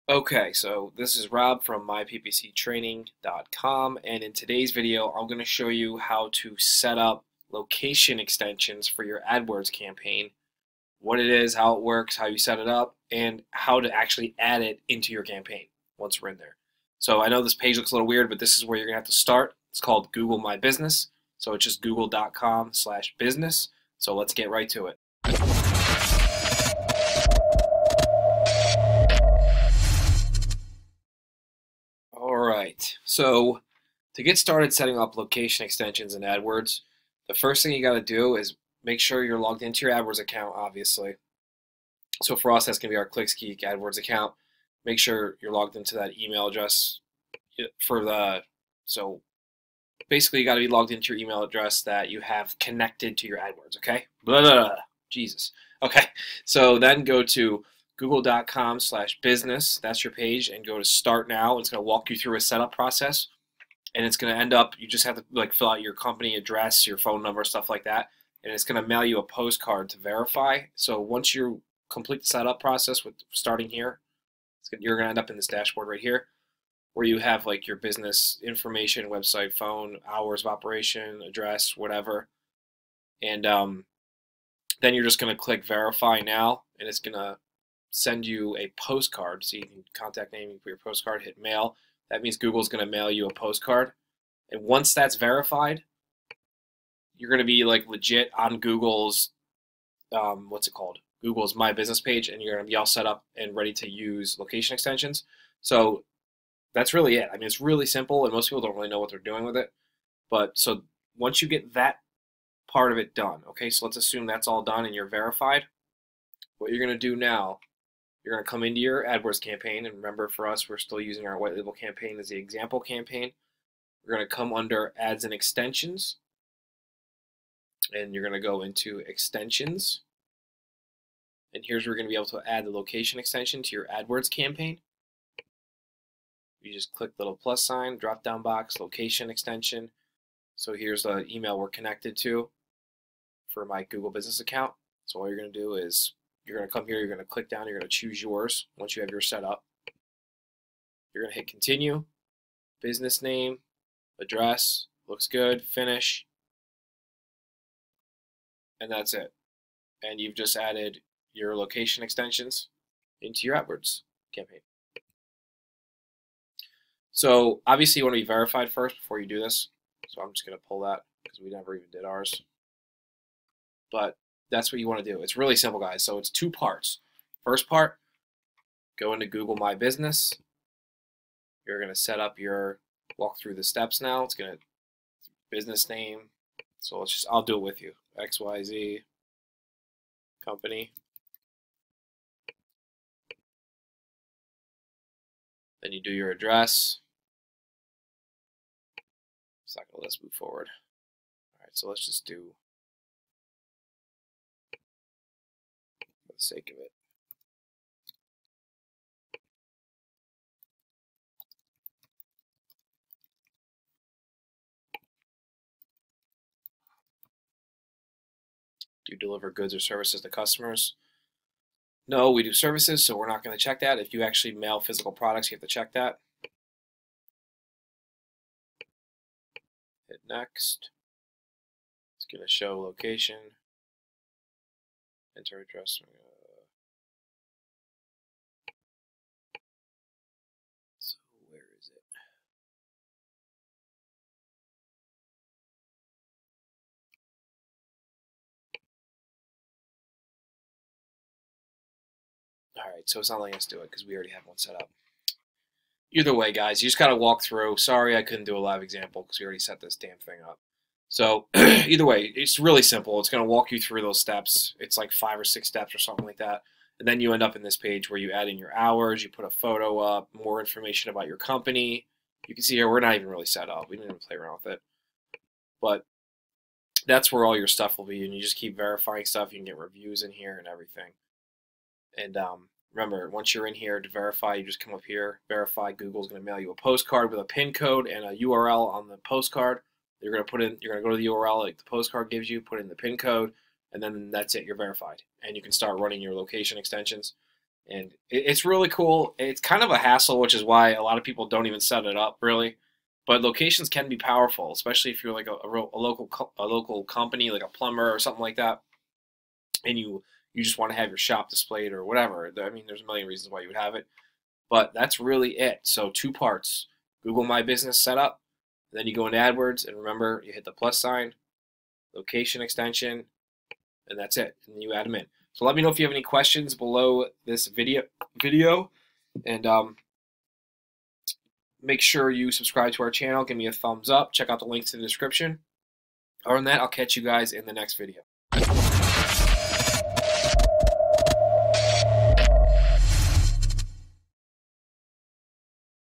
How To Set Up Adwords Location Extentions (Tutorial) - Using The Location Ad Extension
In this training well show you how to set up Adwords location extensions. You'll discover what Adwords location ad extensions are, how they work and how to implement them into your Google Adwords campaign.

⏱️TIMESTAMPS⏱️
0:00 Intro
0:38 Getting Started
0:49 Google My Business
1:45 Adding location extension
5:00 Wrap up & outro